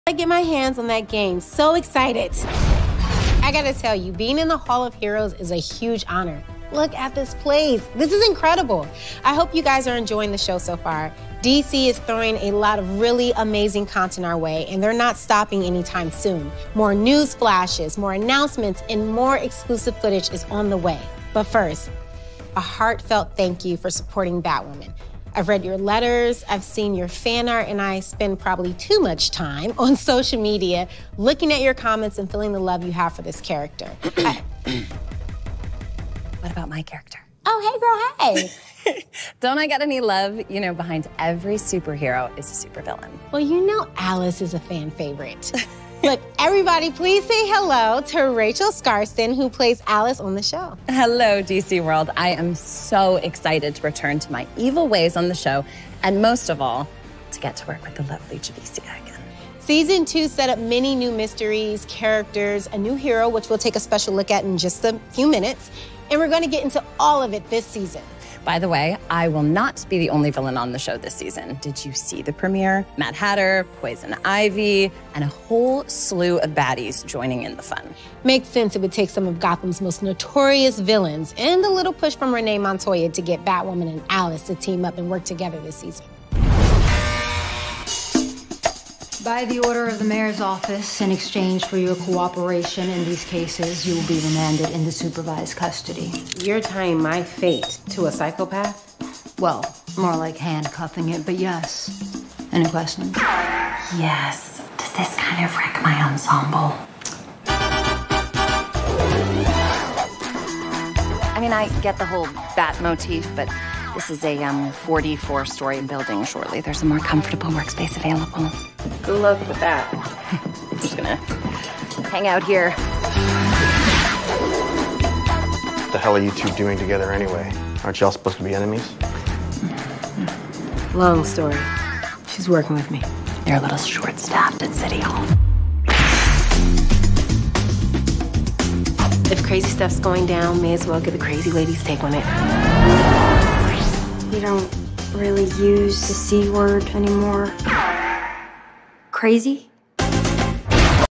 DC Fandome 2021 - Batwoman Panel
"Batwoman" TV series panel featured in DC Fandome 2021.